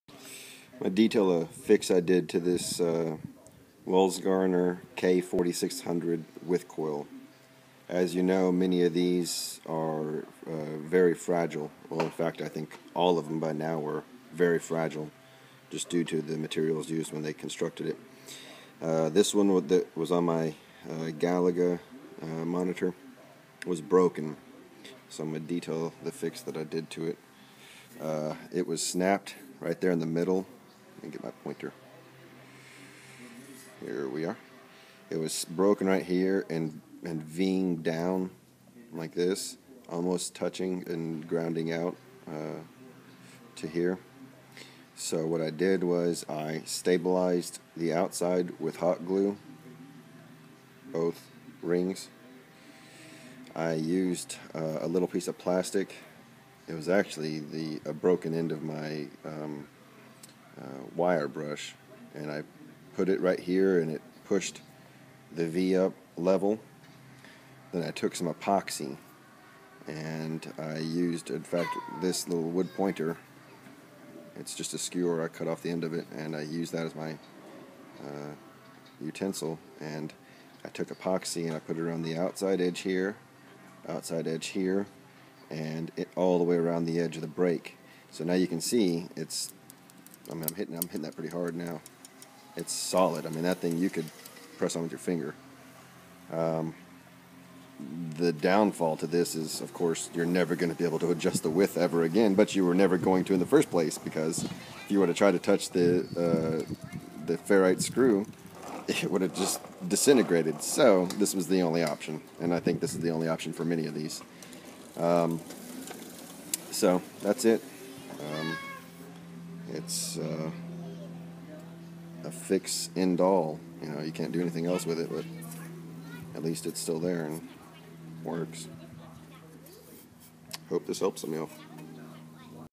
K4600 width coil fix
This fix is a stop gap fix, and will render your coil useless. However, the coil, if broken, is likely useless anyway.